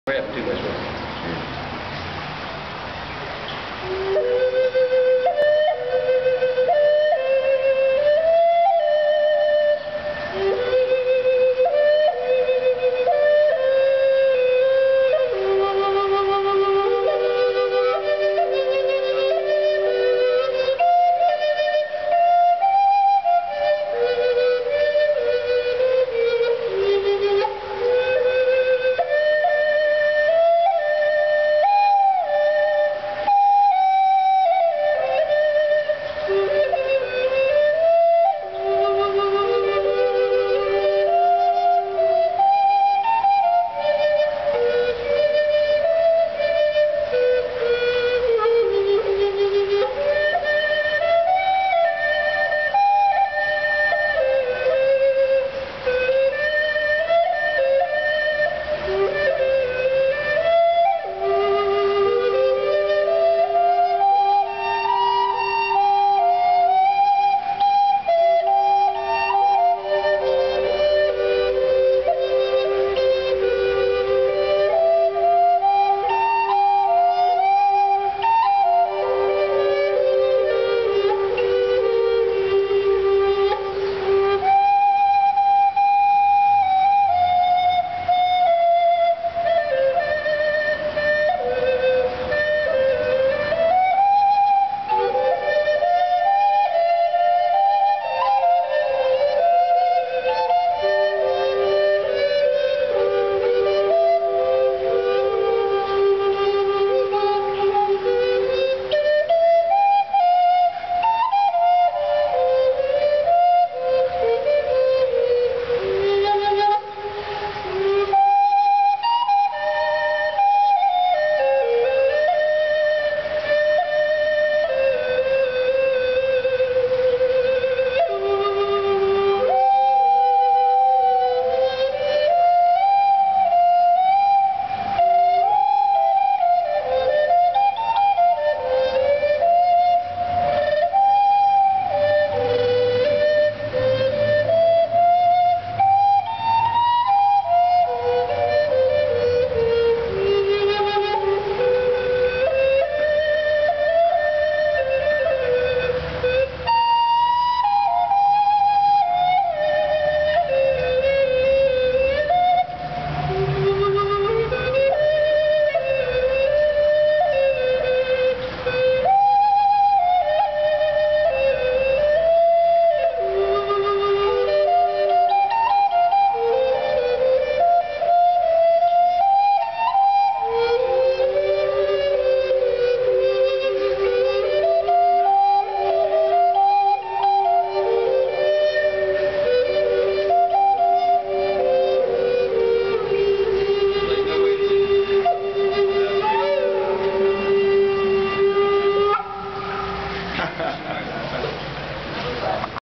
Laughing Crow & Bruce: Flute Improv in Gm
Here are doing our improv thing in G. Pretty cool, eh?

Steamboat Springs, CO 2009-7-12